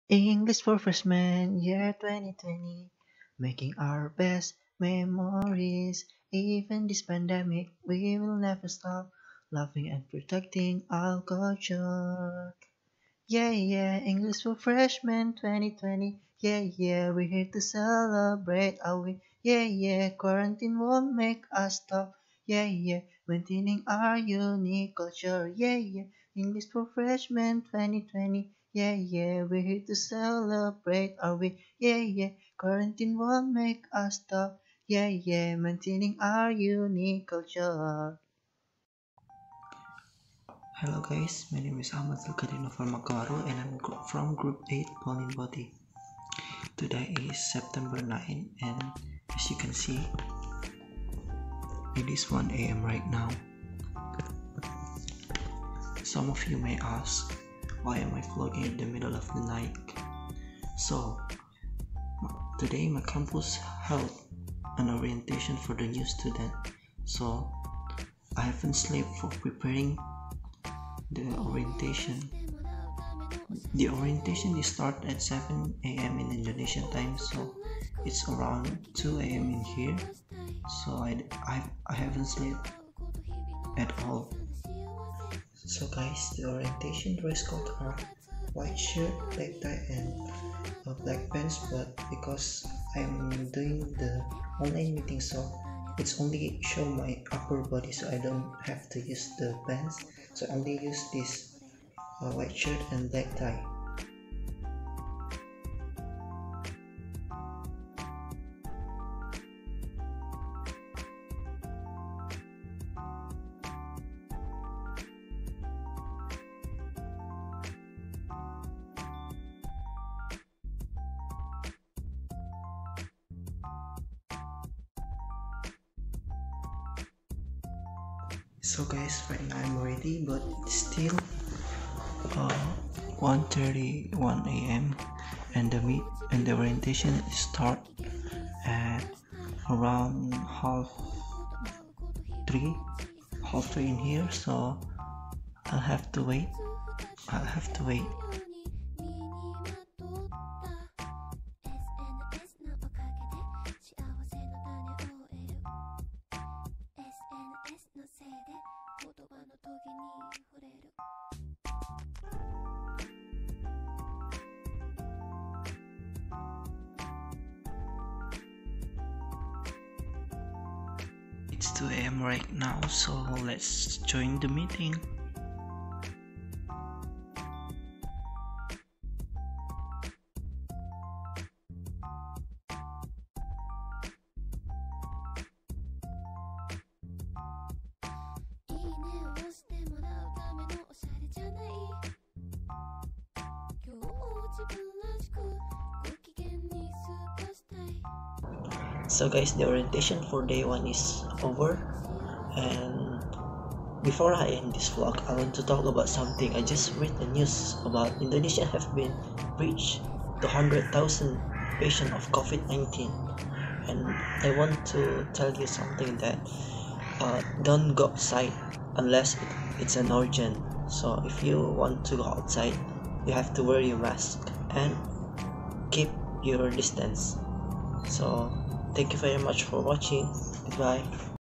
VLOG - Day 1 PKKMB UNY - Orienting from Netherlands
Hello guys! My name is Ahmad Zulqadri Naufal Makkawaru from group 8 Pauline Boty. This vlog is about my first day joining UNY's Orientation from Netherlands. It's quite hard to follow Indonesia's zone time. But, we have to do everything for our university, right?